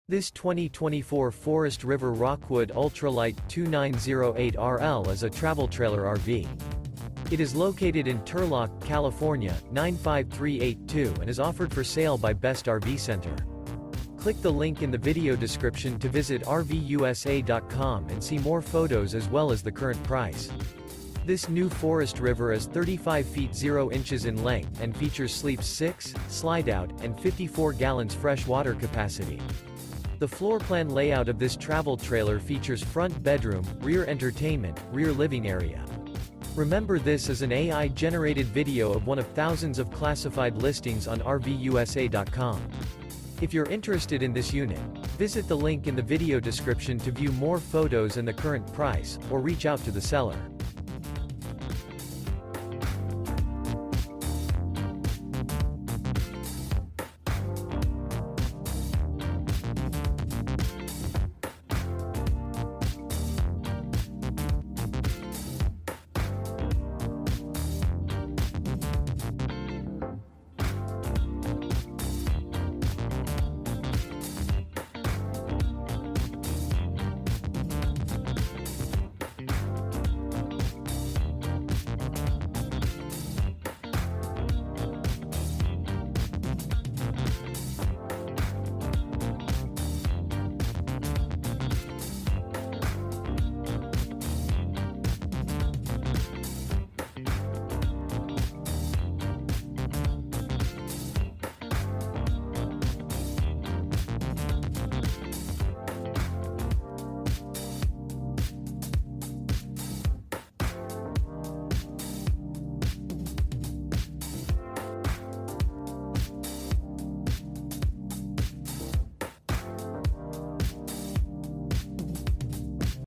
Unbelievable 2024 Forest River Rockwood Ultra Lite Travel Trailer RV For Sale in Turlock, CA
This 2024 Forest River Rockwood Ultra Lite 2908RL is a Travel Trailer RV. It is located in Turlock, CA 95382 and is offered for sale by Best RV Center. This New Forest River Is 35' 0" in length and features sleeps 6, Slideout, and 54 gal fresh water capacity. The floorplan layout of this Travel Trailer features Front Bedroom, Rear Entertainment, Rear Living Area.